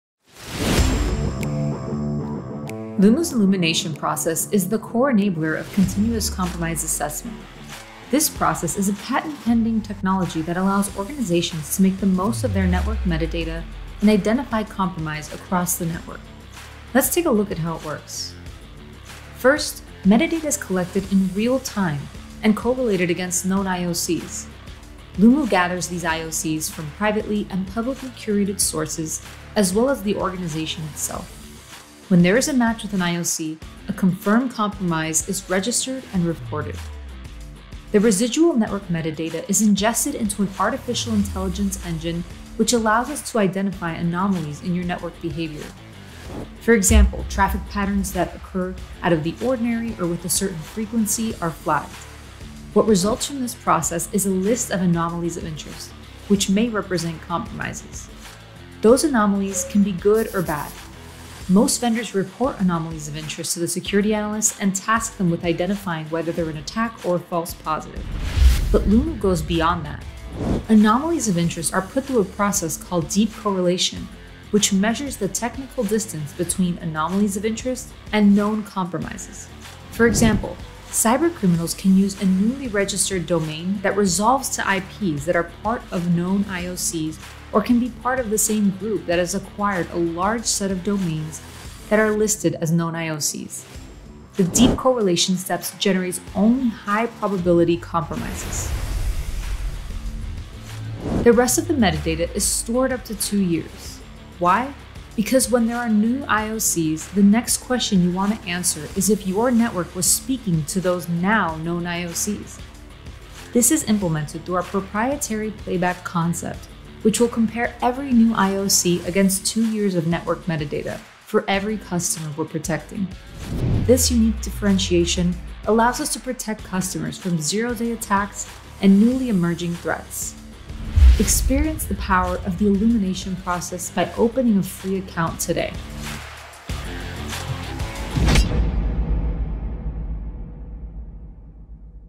Illumination Process
Lumu’s Continuous Compromise Assessment is enabled by our patent-pending Illumination Process. Learn more about how this process uses network metadata and advanced analytics to illuminate your network's dark spots.